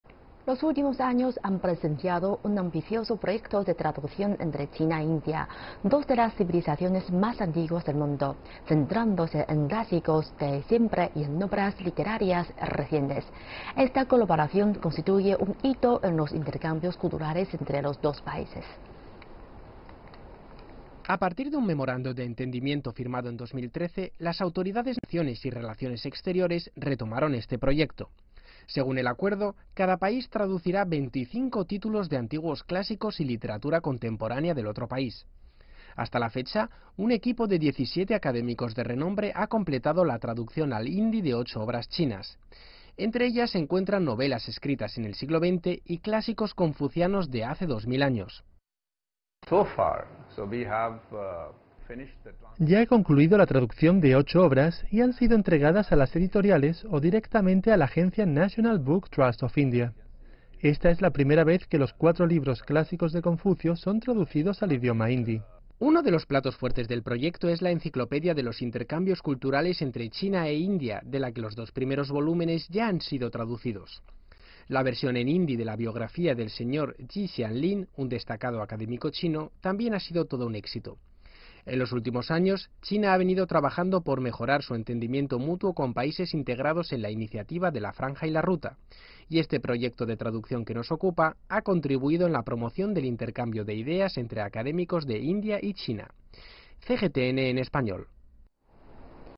Un proyecto de traducción entre China e India facilita el intercambio de antiguas ideas
Los últimos años han presenciado un ambicioso proyecto de traducción entre China e India, dos de las civilizaciones más antiguas del mundo. Centrándose en clásicos de siempre y en obras literarias recientes, esta colaboración constituye un hito en los intercambios culturales entre los dos países.